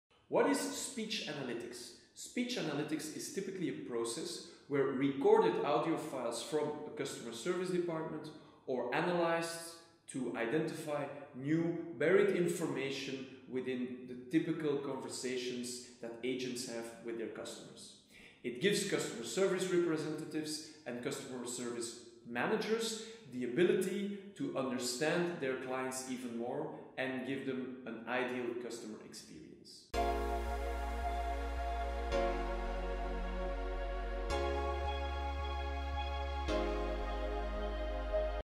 What is Speech Analytics? 🗣| iReachm
New serie: "What is..?"
Made to educate you about the evolution of technology.

In this episode: What is Speech Analytics?